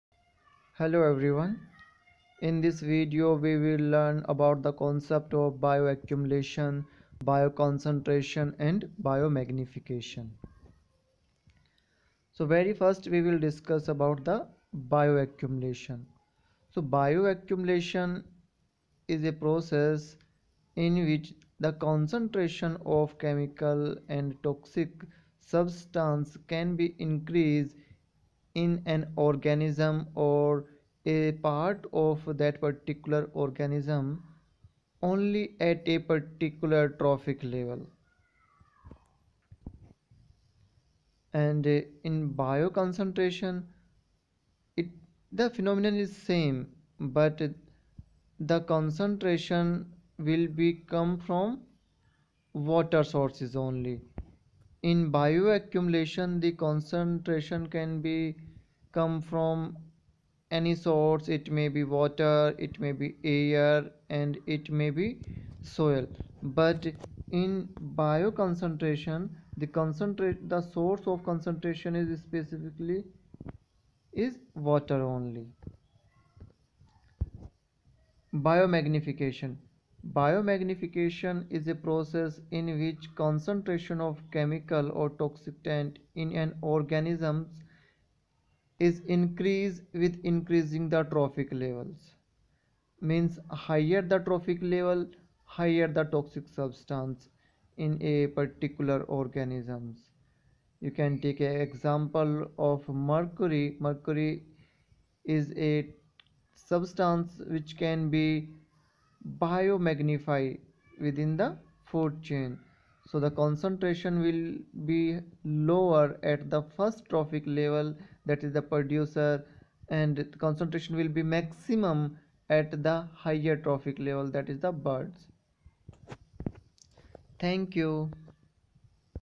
BIOACCUMULATION| BIOCONCENTRATION| BIOMEGNIFICATION
Bioaccumulation is defined as  the increase in concentration of chemical/toxicant in an organism or a part of that organism ONLY at a particular trophic level
Bioconcentration is the accumulation of a chemical/toxicant  in or on an organism when the source of chemical/toxicant is  WATER ONLY
Biomagnification is the increase in the concentration of chemical/toxicant as you go higher in a food chain